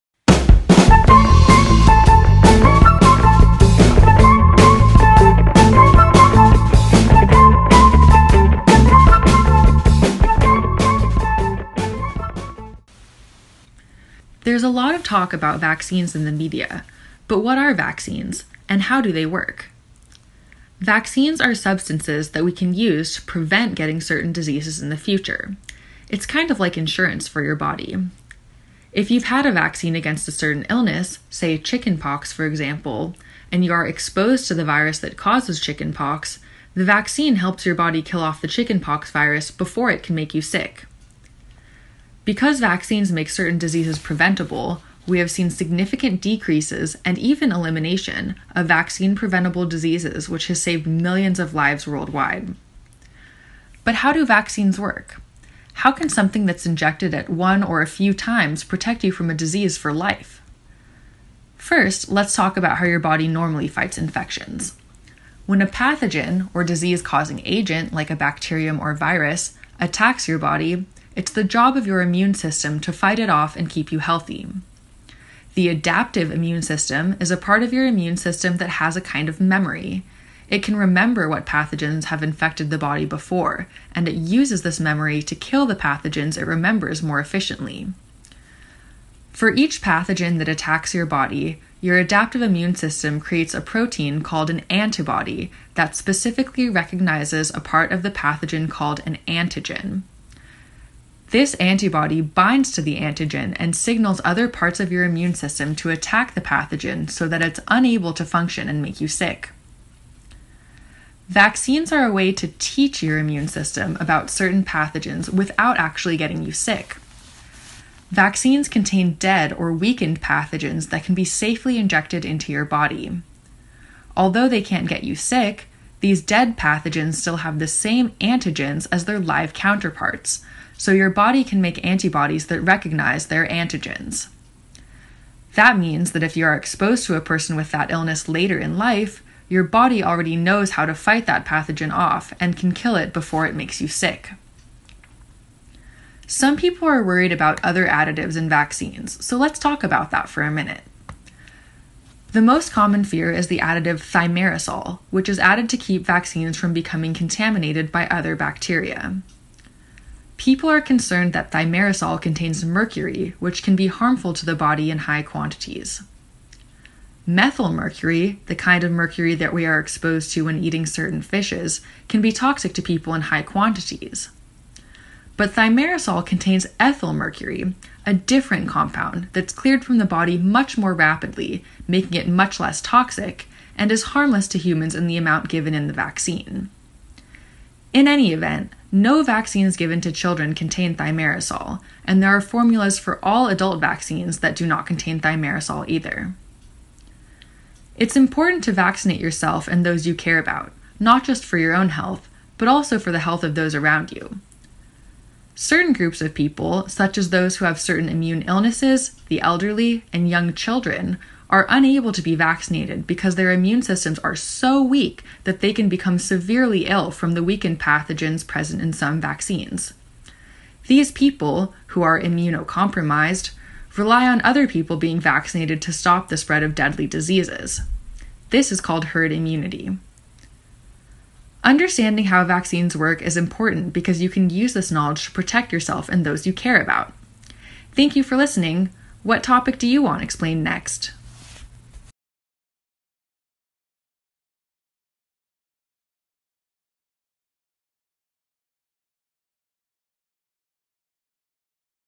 What are vaccines?
Ever wonder how vaccines work and what they're doing in your body? I break it down for you in just 4 minutes!